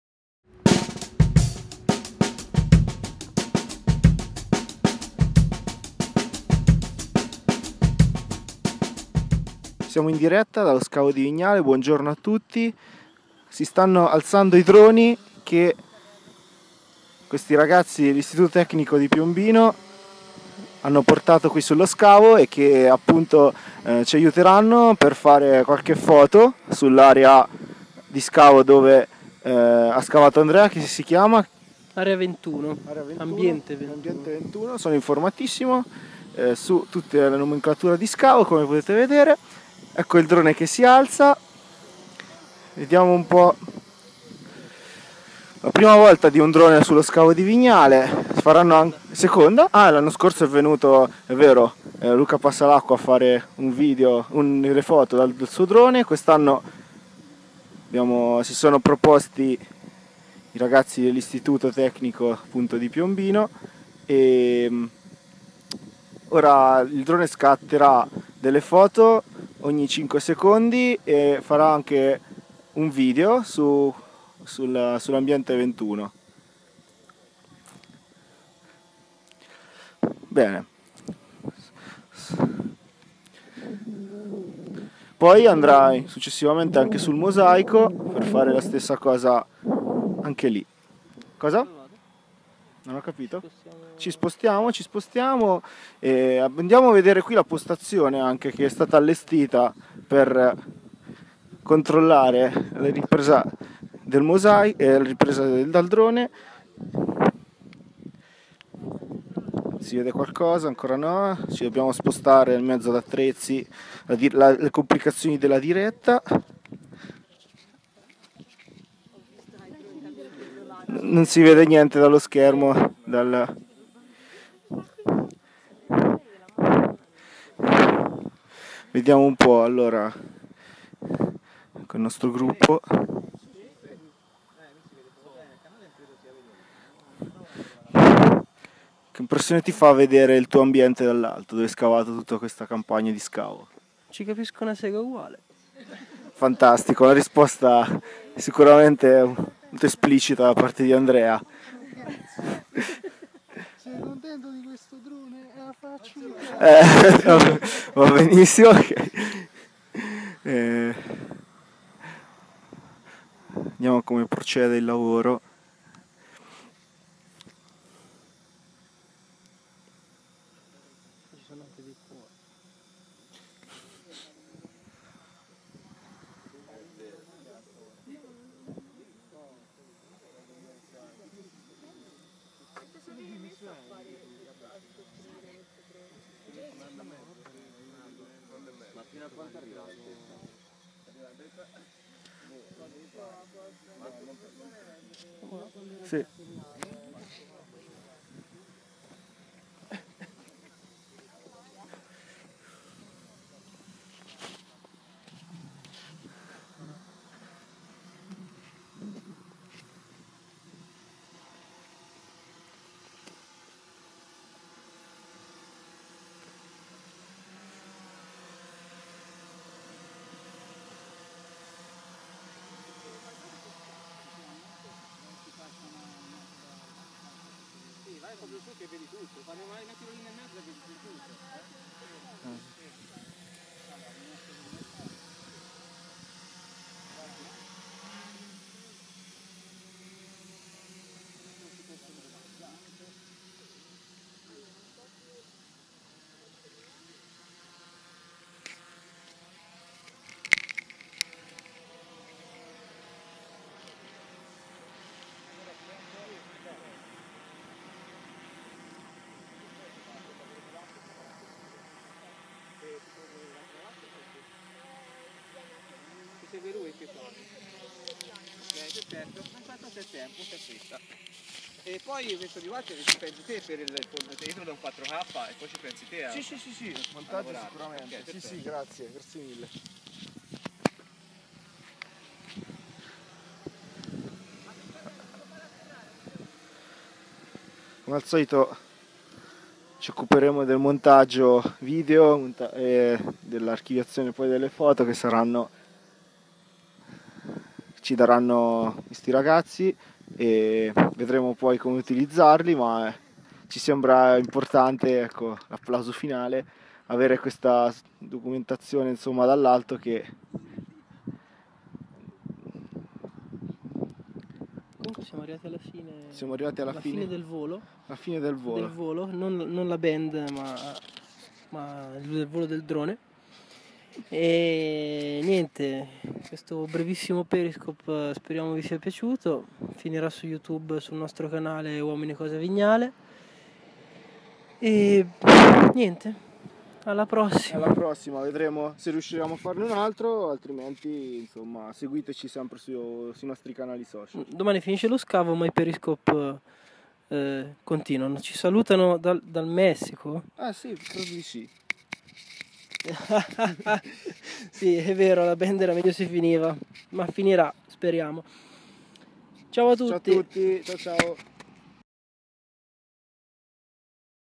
Si alza il drone
La diretta Periscope dei droni in azione sullo scavo, a cura dell'IPSIA di Piombino